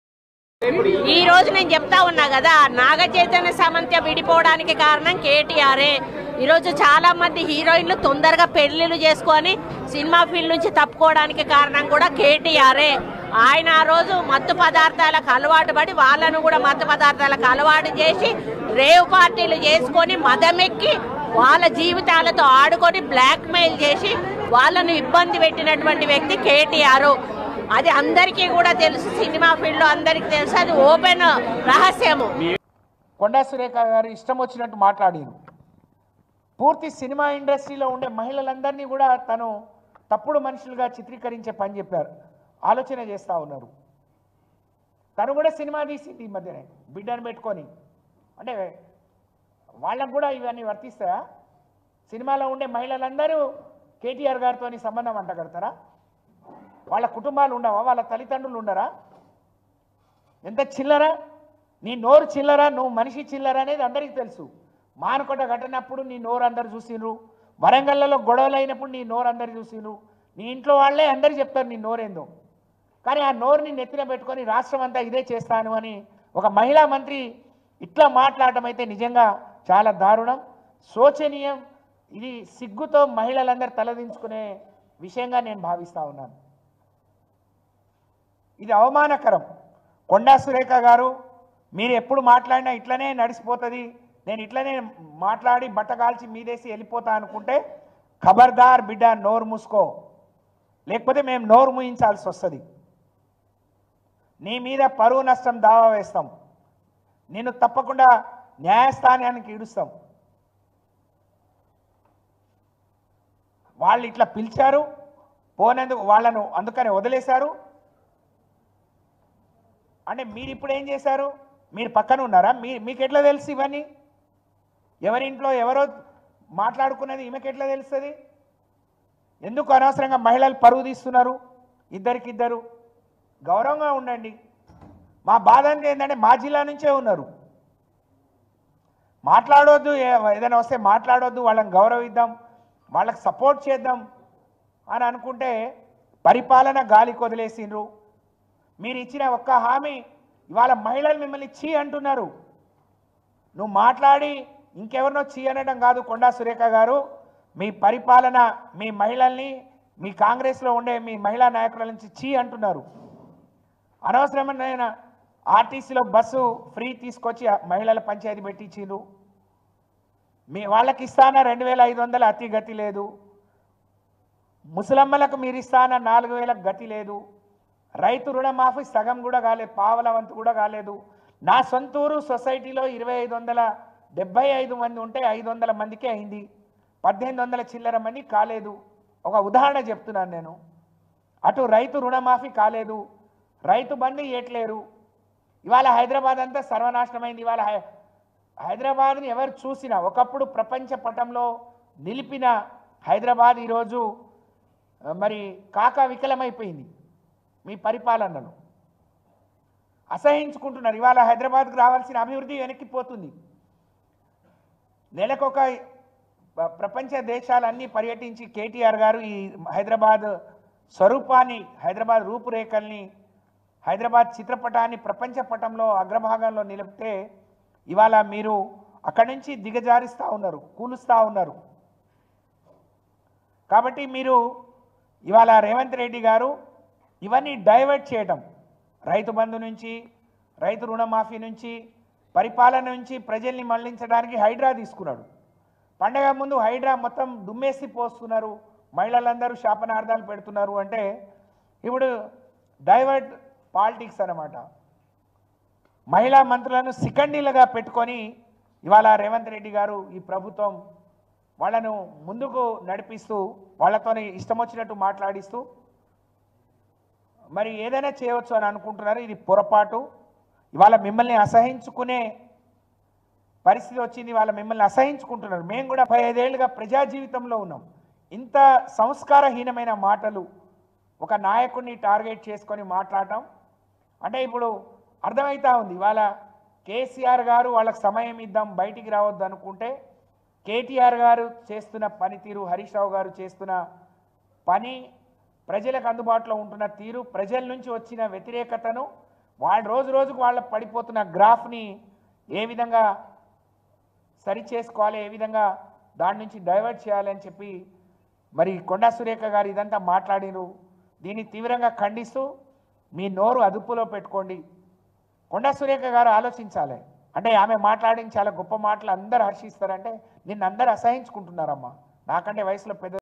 Ex Minister Satyavathi Rathod Reaction on Minister Konda Surekha Comments on KTR | Samantha
Ex Minister Satyavathi Rathod Reaction on Minister Konda Surekha Comments on KTR | Samantha @iDreamKhammam